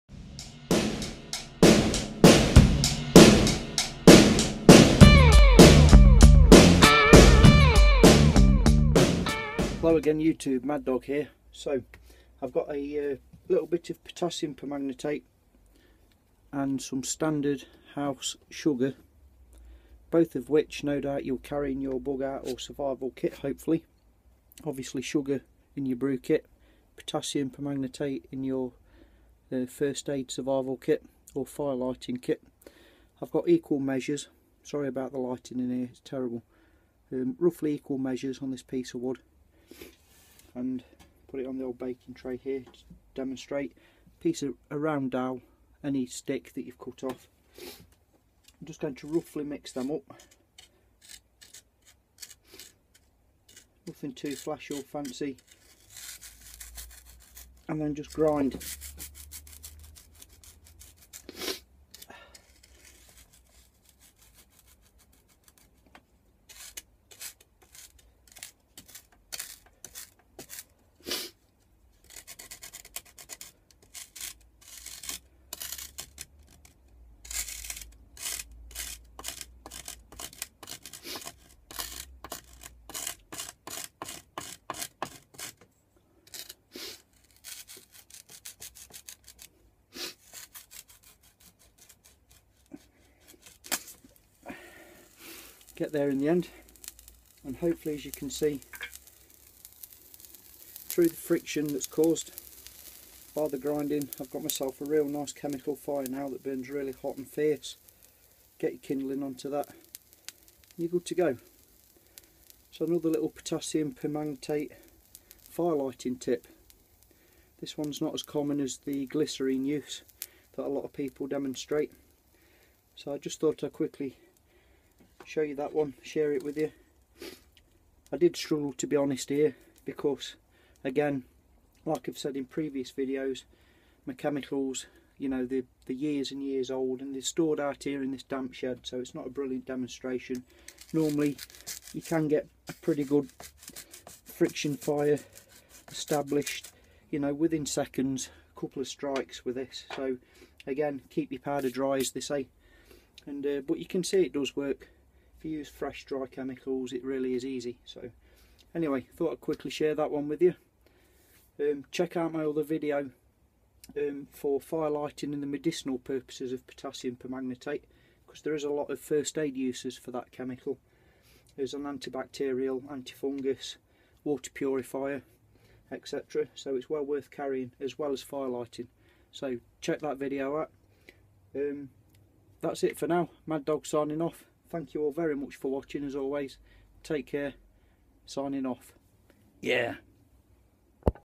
Fire from sugar and potassium permanganate!
Chemical fire using sugar potassium permanganate and a bit of friction!